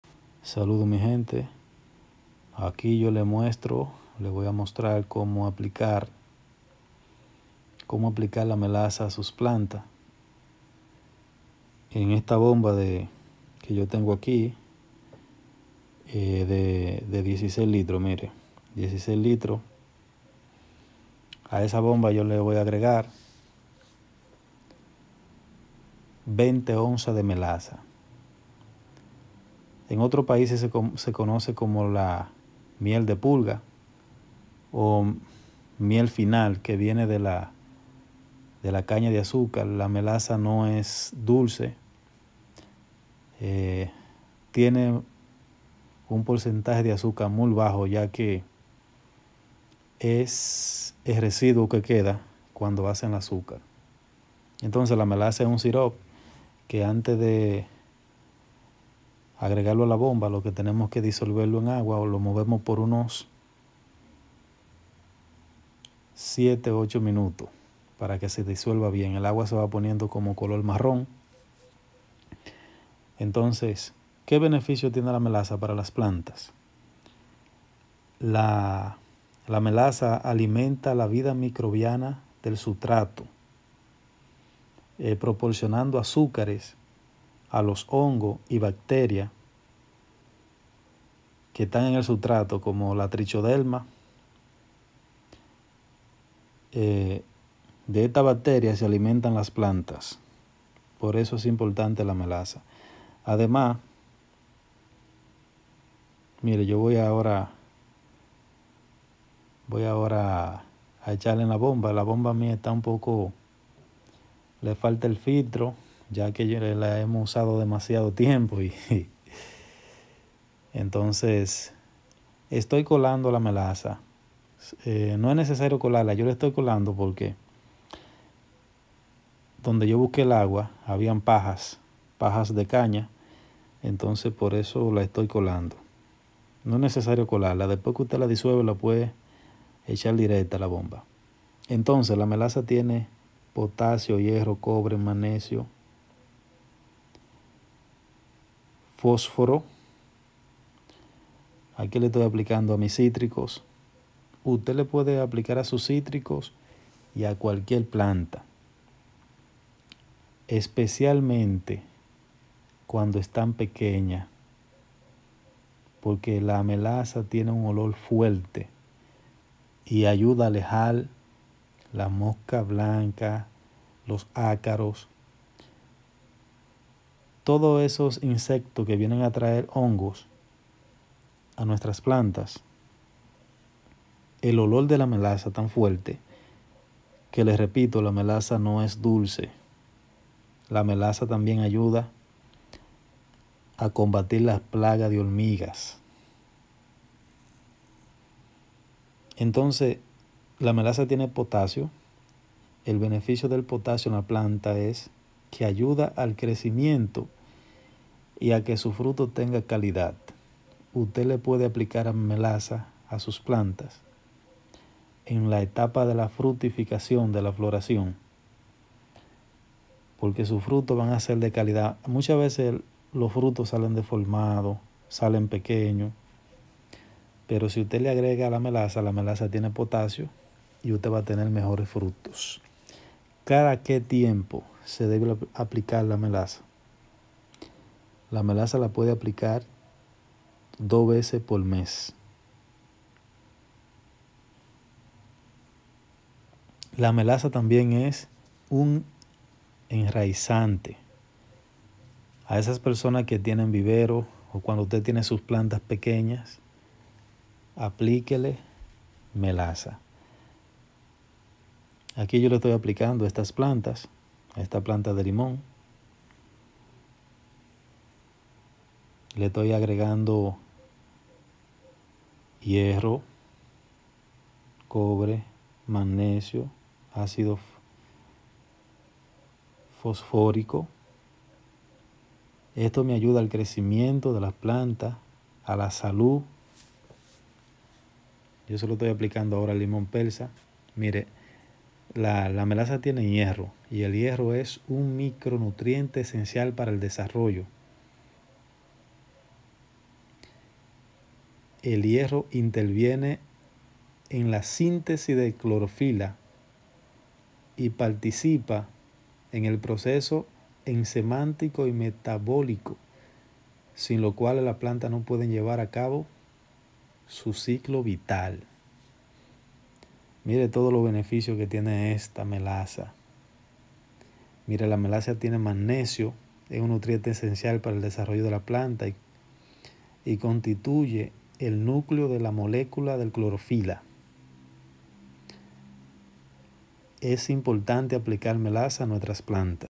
El Fertilizante Orgánico 100 veces más potente para las plantas
Aquí te mostraremos un fertilizante orgánico que te ayudará a tener una mejor cosecha .

Aplicación de la melaza :
1- A una bomba de 16 litros le agrega 20 onzas de melaza disuelta en agua .

2 -Se aplica a las plantas desde sus nacimiento hasta la fructificación.